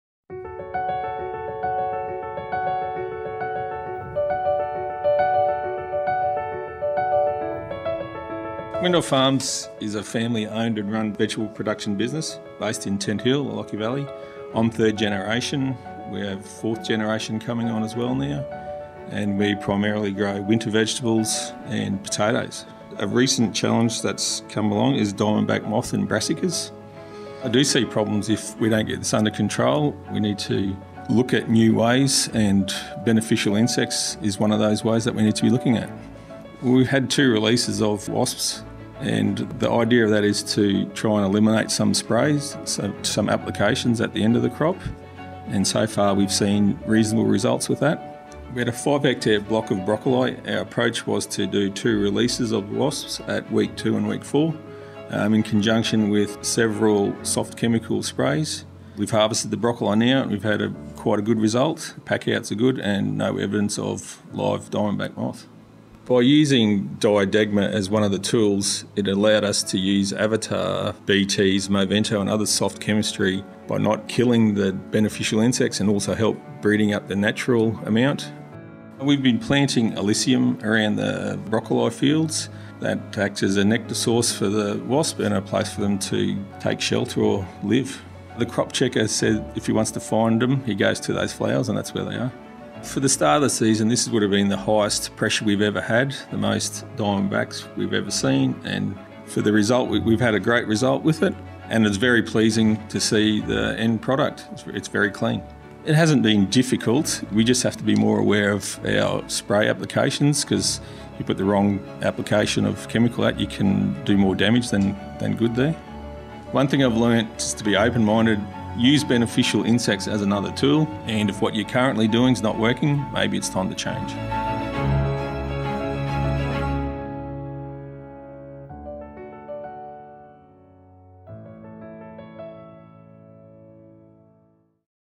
Paul Windolf –Diadegma and Movento® for Integrated Diamondback Moth Management in Broccoli
Paul Windolf, from Windolf Farms in the Lockyer Valley, shares his experience and approach to managing diamondback moth in broccoli using an integrated pest management approach which includes diadegma wasp releases along with selective chemistry like Movento 240 SC Insecticide.